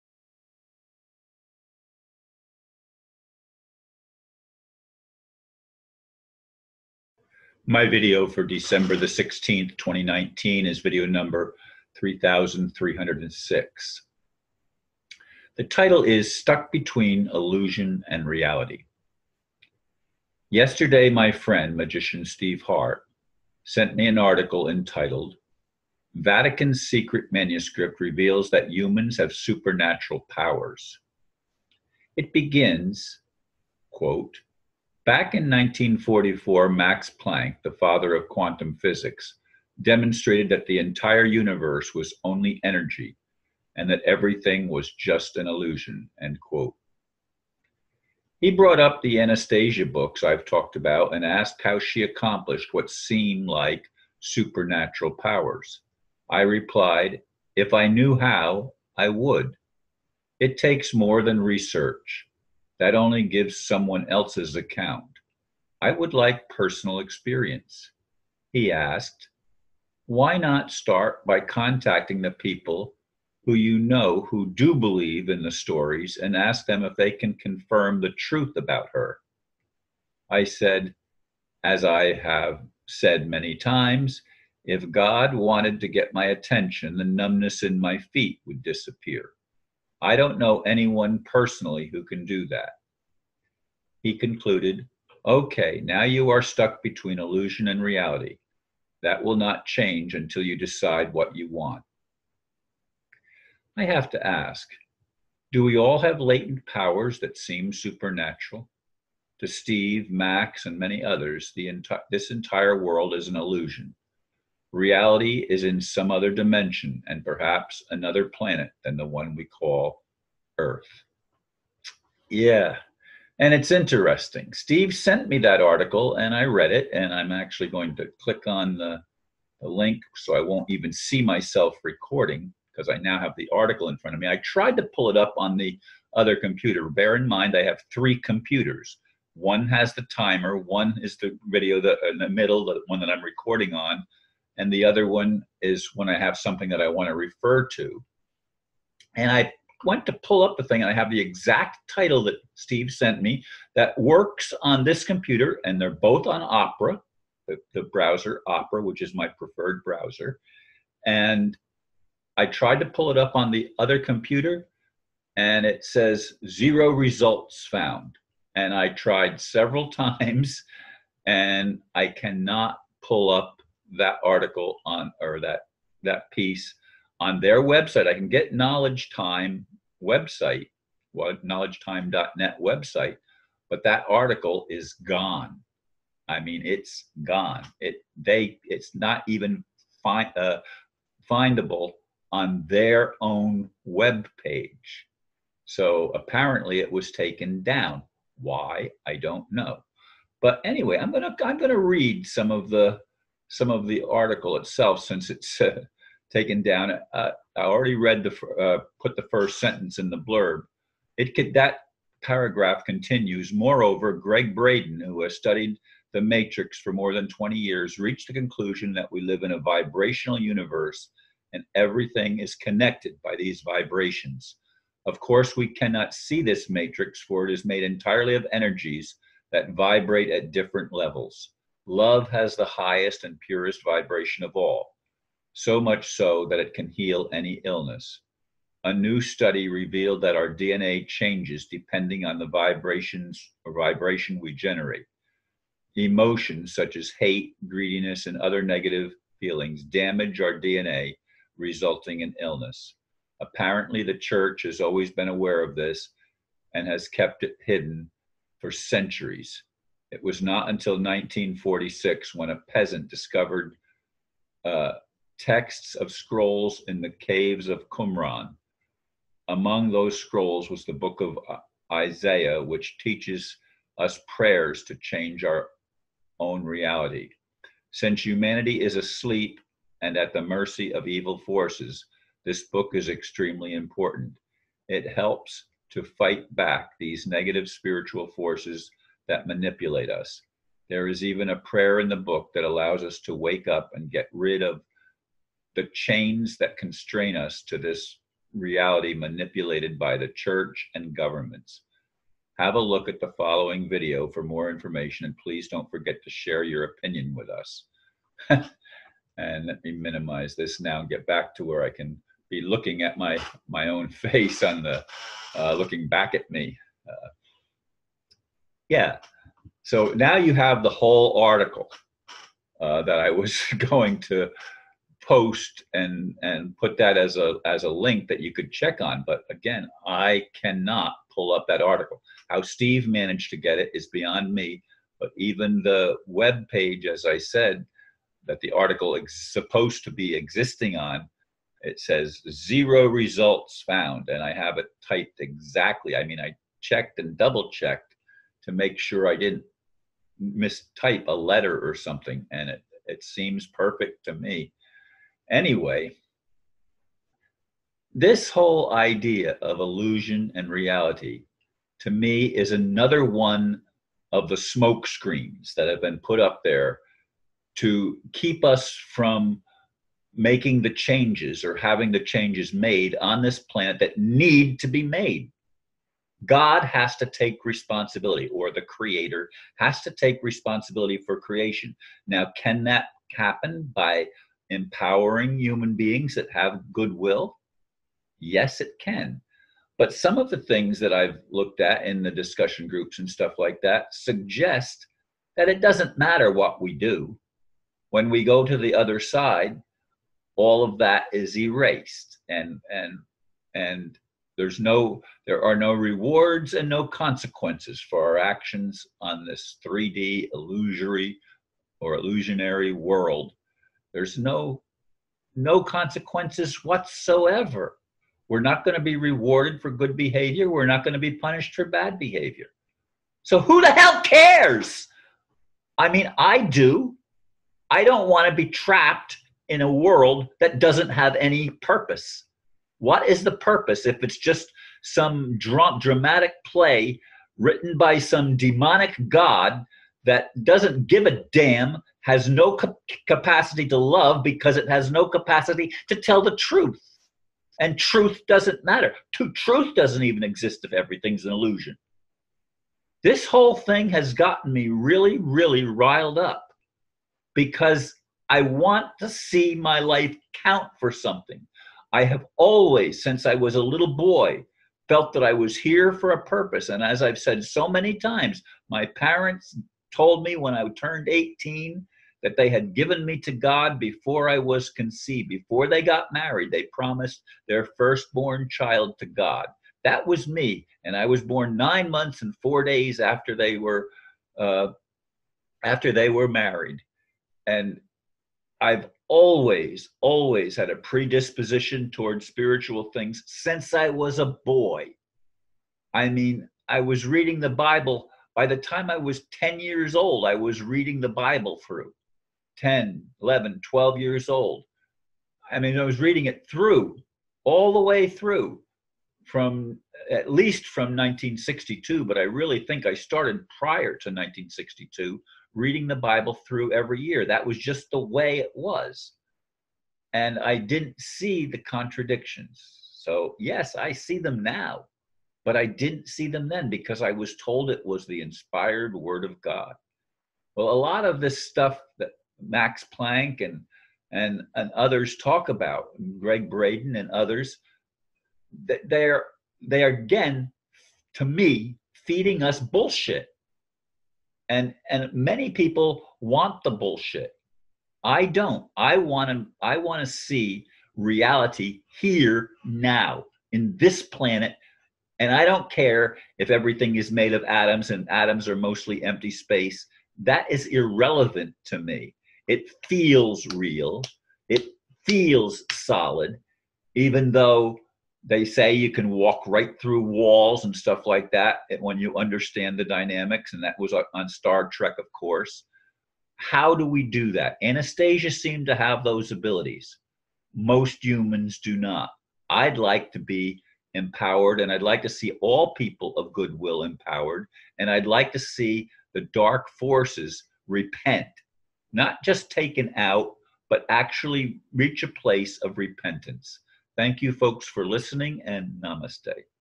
12-16-19 – STUCK BETWEEN ILLUSION AND REALITY
STUCK BETWEEN ILLUSION AND REALITY
Yesterday my friend, magician Steve Hart, sent me an article entitled: Vatican’s Secret Manuscript Reveals That Humans Have Supernatural Powers. It begins: “Back in 1944, Max Planck, the father of quantum physics, demonstrated that the entire universe was only energy and that everything was just an illusion.” He brought up the Anastasia books I've talked about and asked how she accomplished what seem like supernatural powers. I replied: "If I knew how, I would. It takes more than research. That only gives someone else's account. I would like personal experience." He asked: "Why [not] start by contacting the people who you know who do believe in the stories and ask them if they can confirm the truth about her." I said: "As I have said many times, if God wanted to get my attention, the numbness in my feet would disappear. I don't know anyone personally who can do that." He concluded, "OK, now you are stuck between illusion and reality; that will not change until you decide what you want." I have to ask: Do we all have latent powers that seem supernatural? To Steve, Max and many others, this entire world is a illusion. Reality is in some other dimension and perhaps another planet than the one we call: Earth.